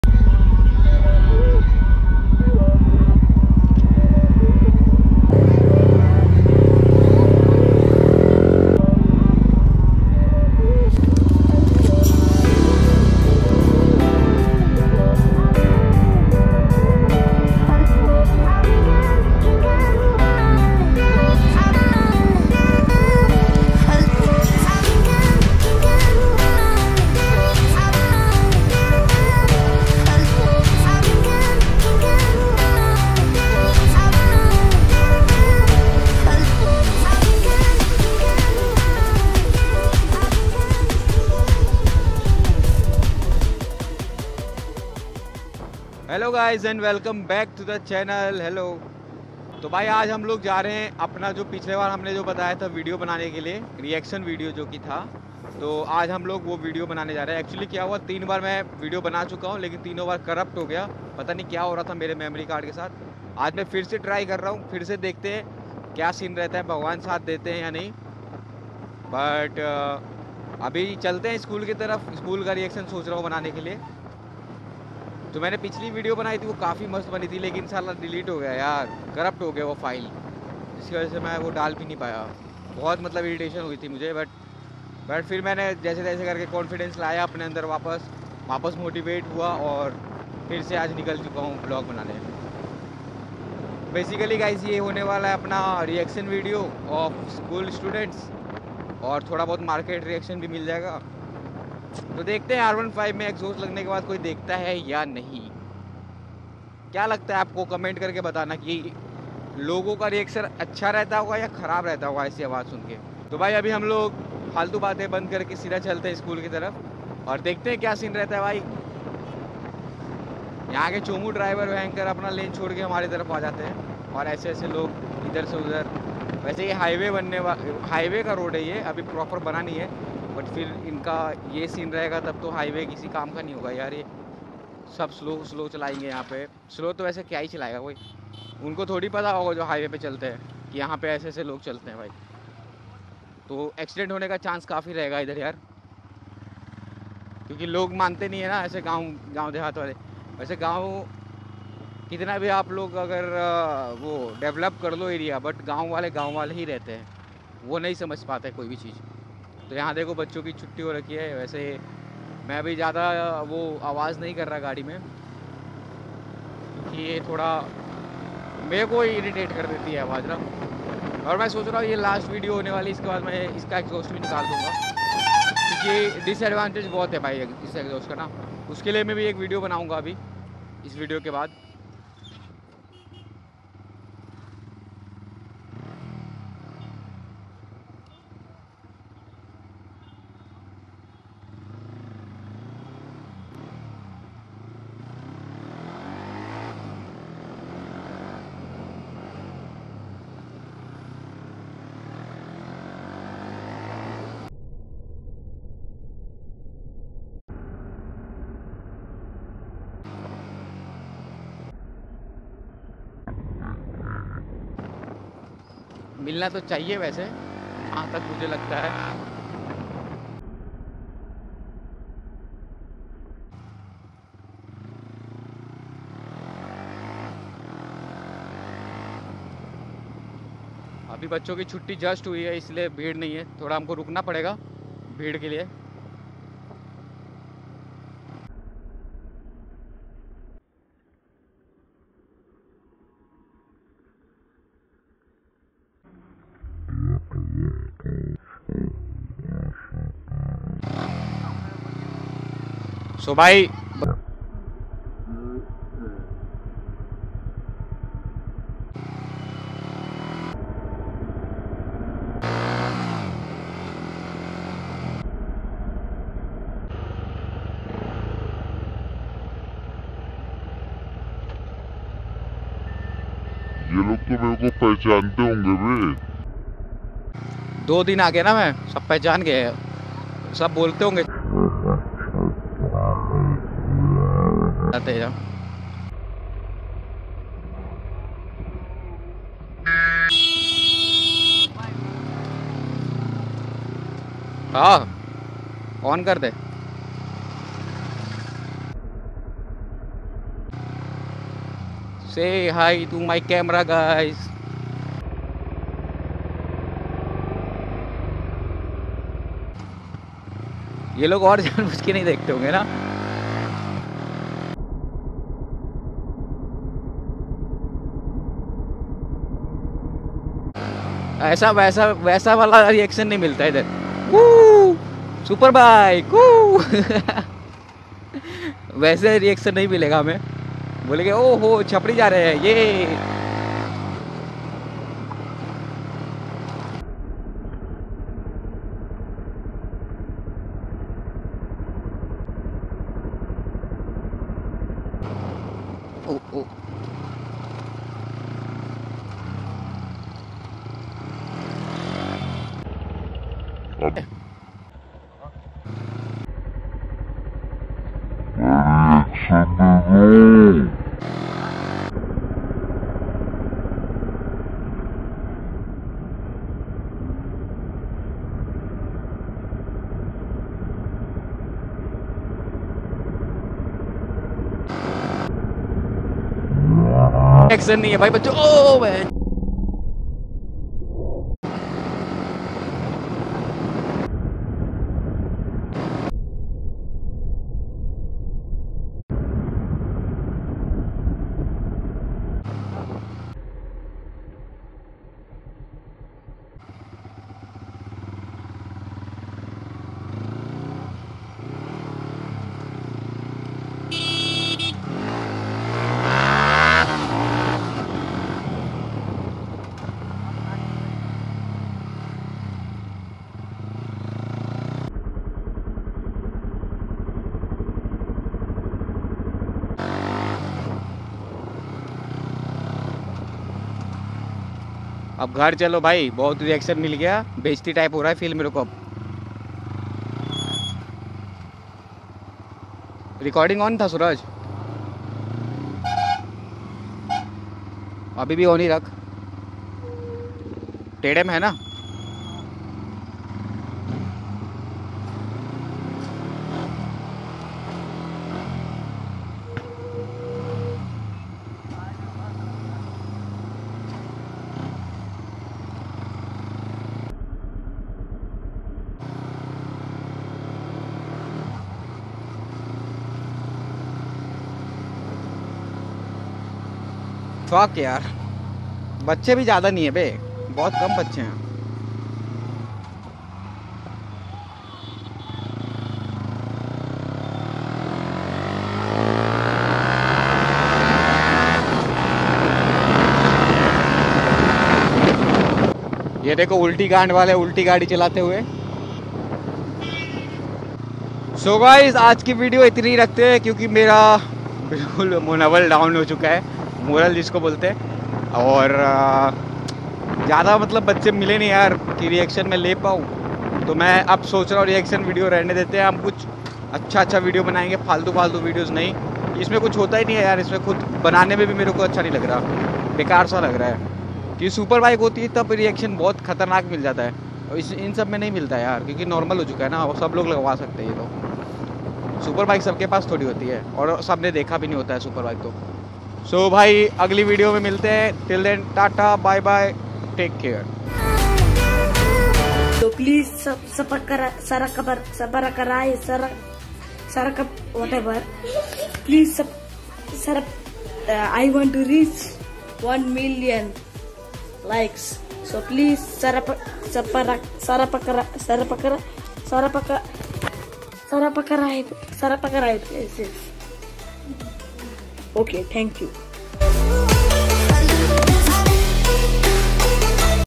Cute School Girls Reaction on My Superbike | R15V3 😍😍😍
Revving Hard My Modified R15 V3 | School Students Cute Reactions 😍😍